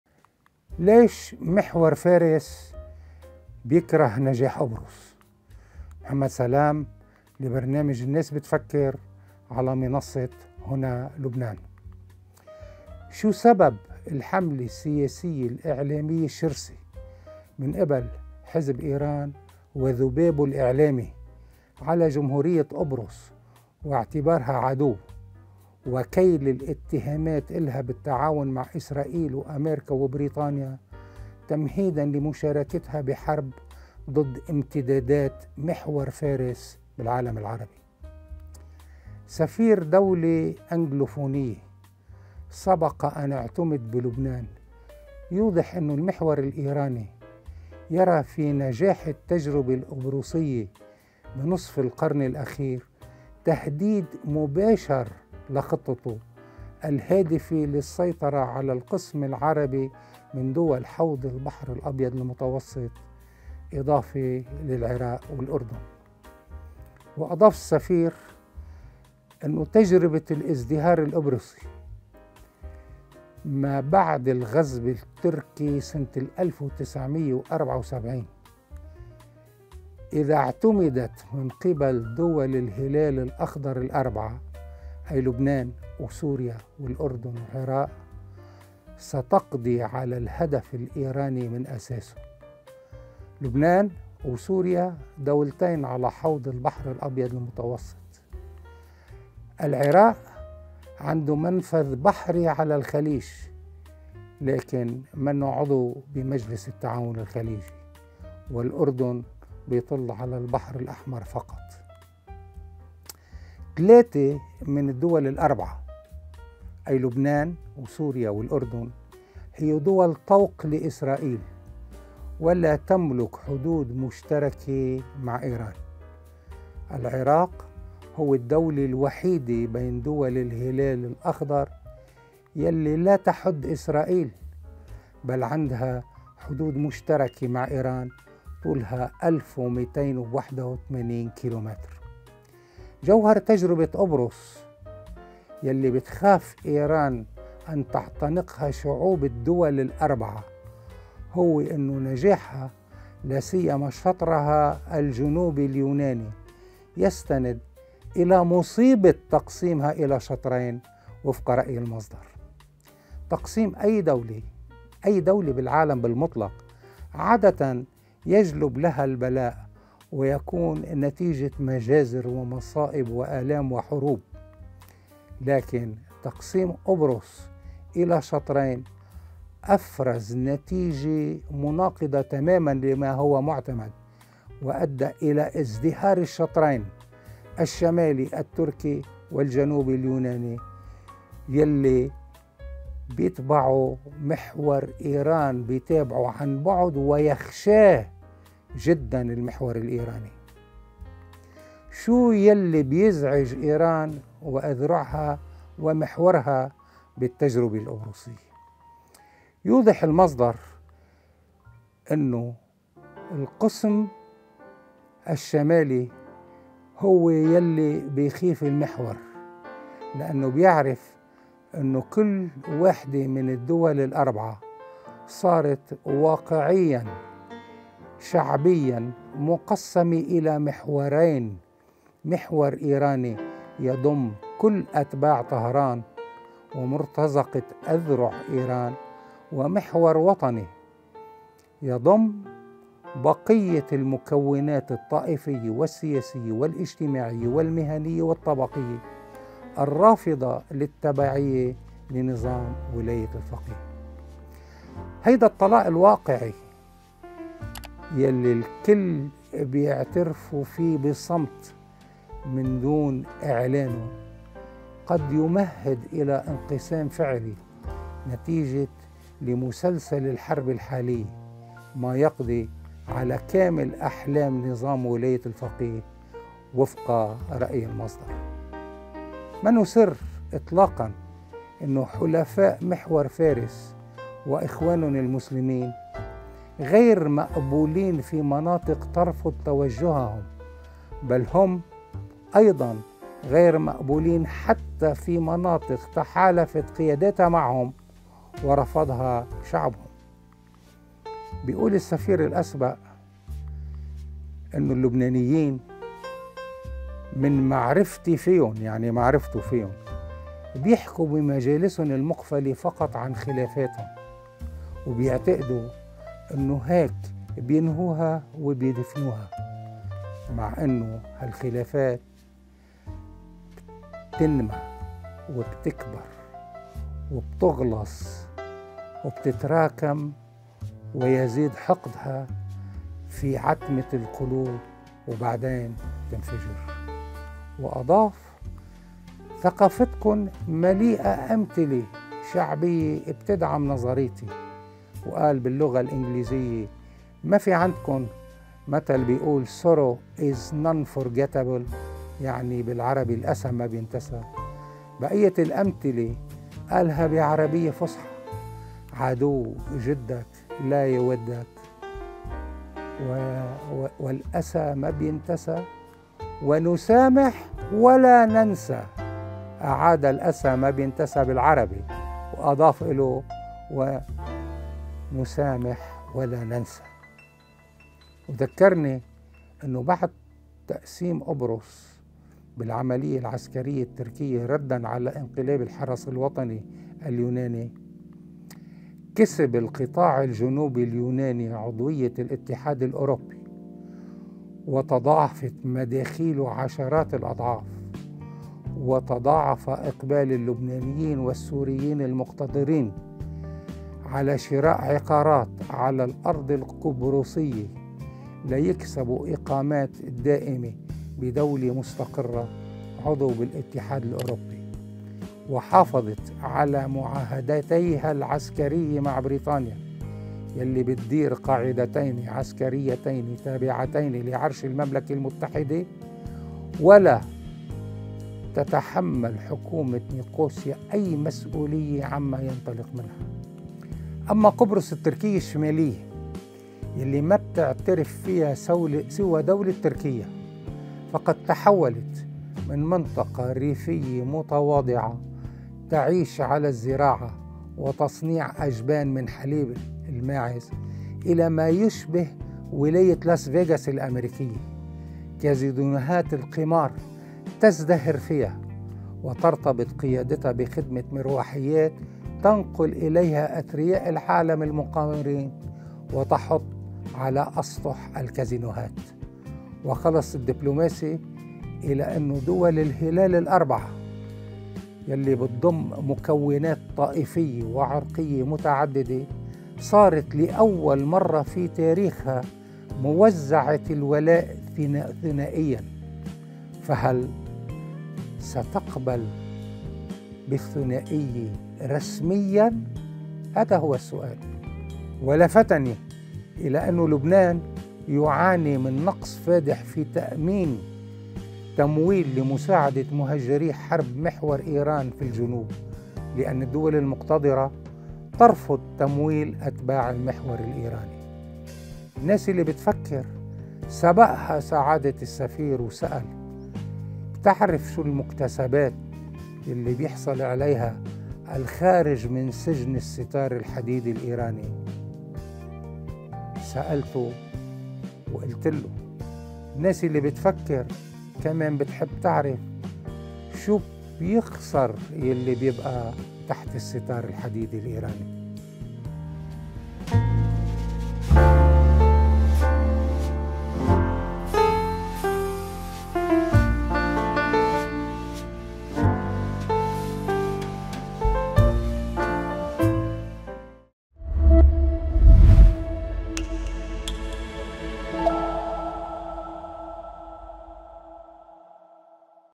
هل يكره محور "فارس" نجاح قبرص؟‎
في هذه الحلقة من “الناس بتفكر”، يتوقف الصحافي محمد سلام عبر منصة "هنا لبنان" عند حملة "الممانعة" التي تستهدف قبرص، فماذا قال عن مواقفهم من التقسيم؟ وكيف قيّم واقع إيران في المنطقة؟